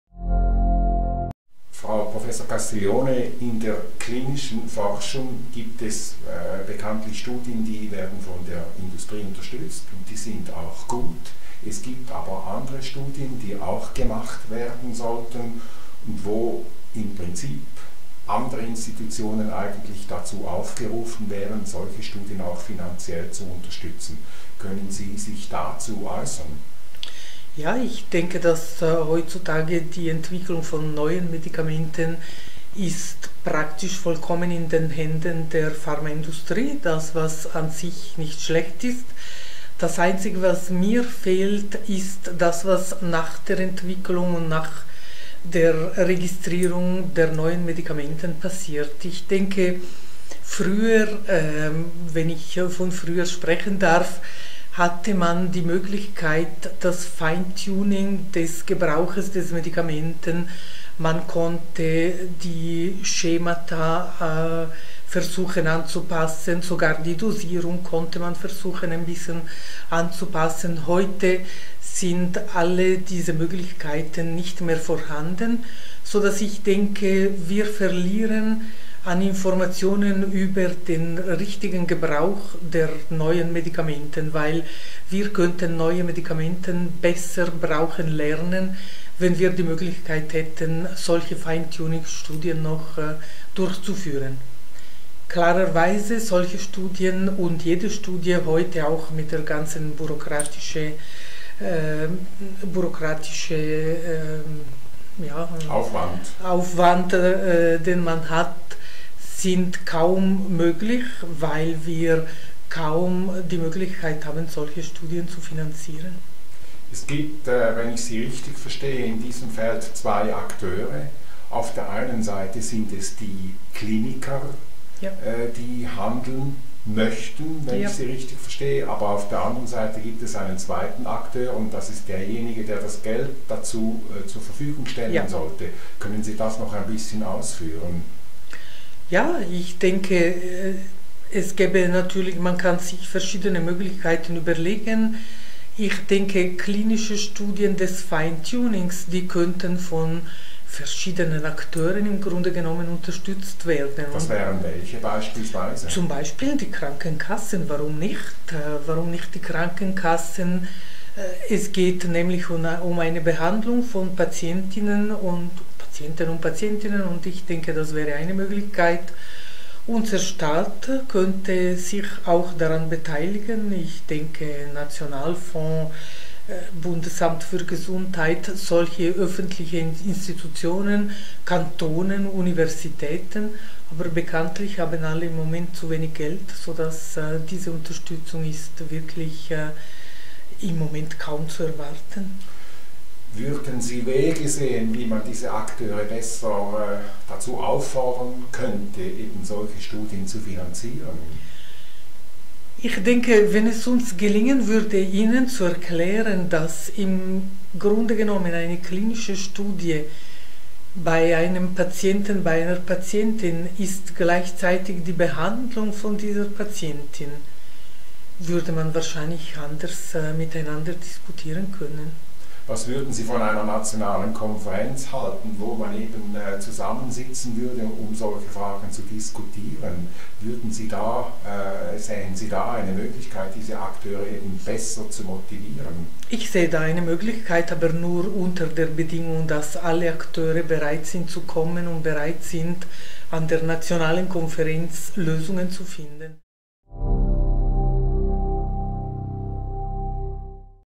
Prof. Dr. med. Monica Castiglione-Gertsch im Interview zur Finanzierung unabhängiger Studien
Prof. Dr. med. Monica Castiglione-Gertsch im Interview zur Finanzierung unabhängiger patientenzentrierter Studien